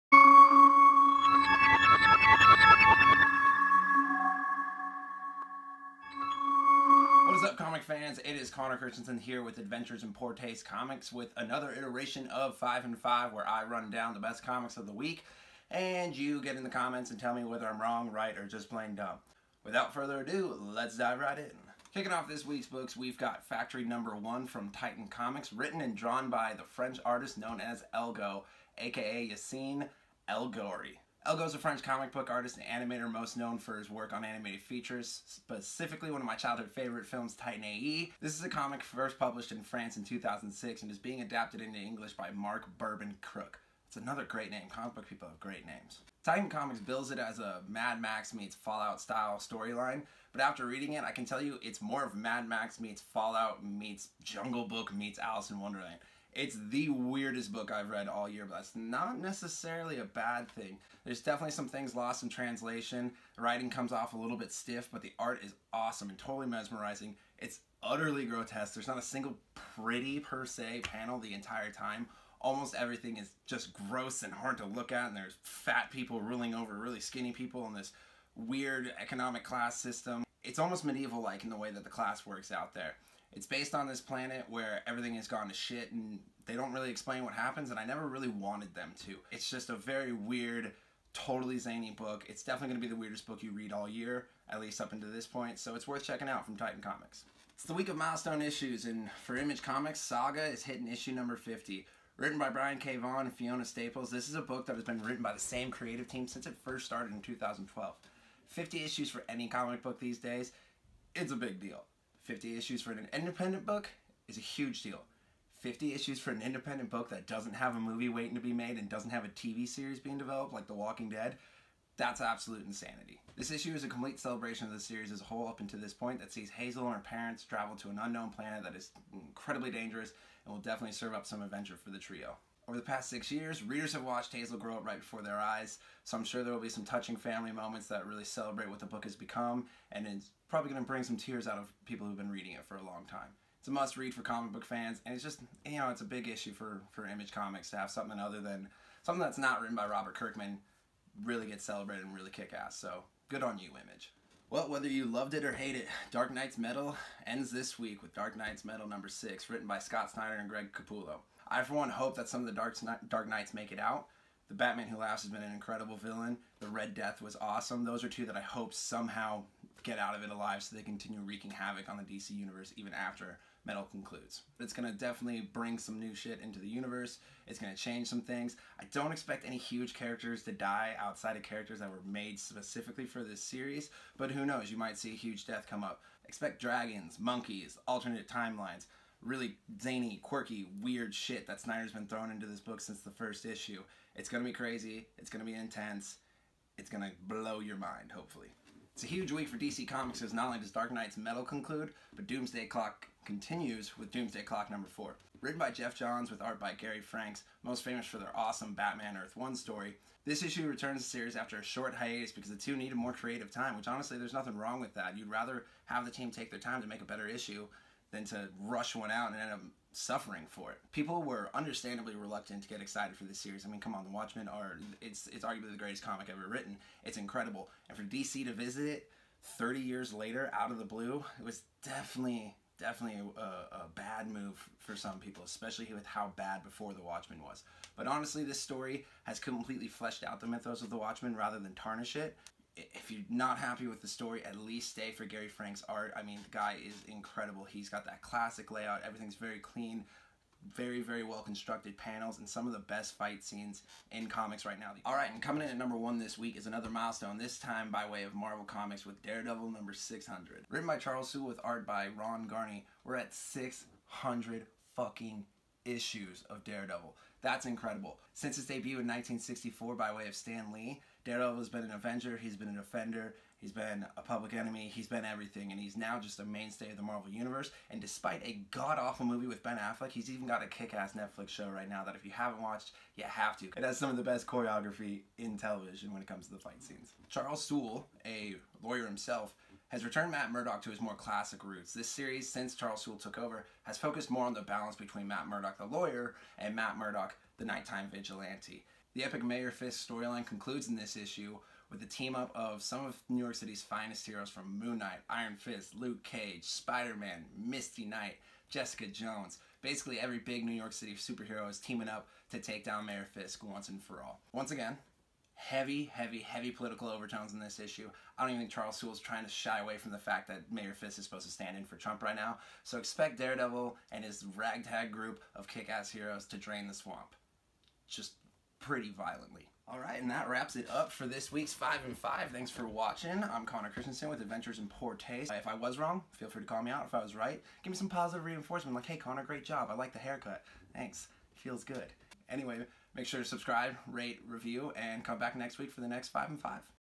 5 in 5: March 28, 2018 – Daredevil #600, Saga #50, and more
Our most anticipated comic books the week of March 28, 2018.

#3 – Dark Nights: Metal #6

#2 – Doomsday Clock #4

Find more reviews and news for toys, comic books, cosplay and more on our